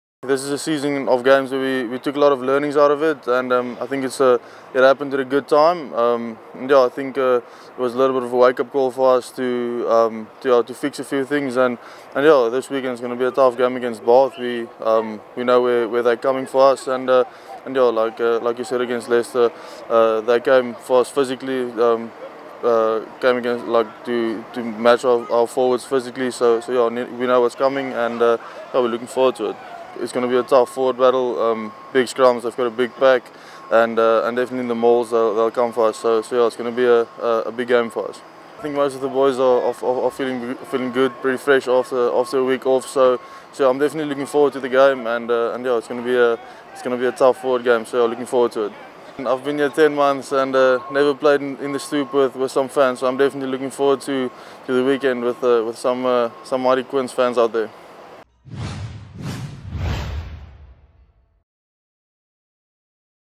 Wilco Louw previews Bath clash
Wilco Louw says defeat against Leicester came at a good time as the squad has been able to fix a few things with a weekend off.